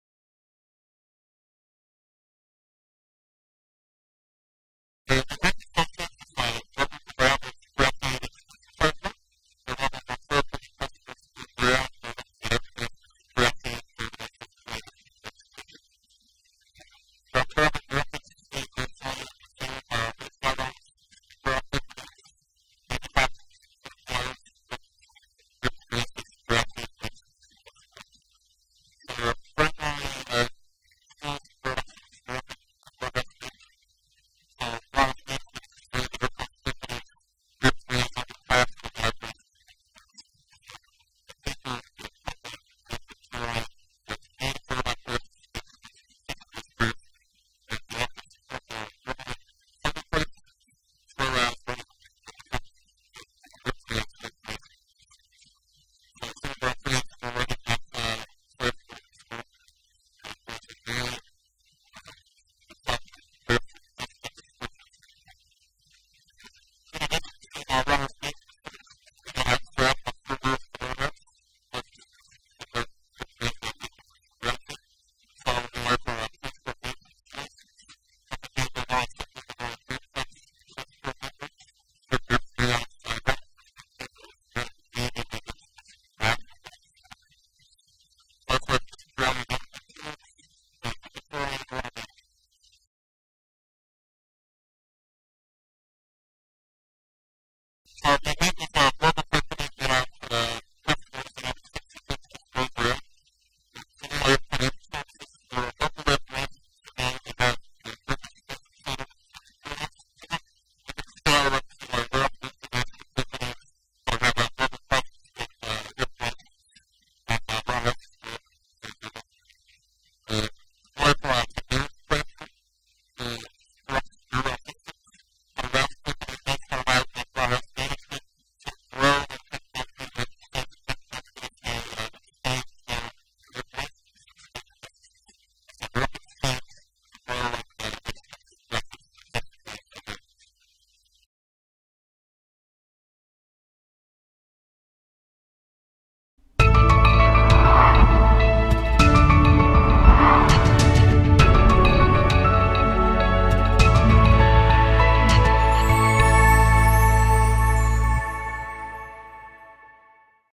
Manhattan Associates talks to Global Logistics Media
Russ from Manhattan Associates talks to Global Logistics Media. Video courtesy of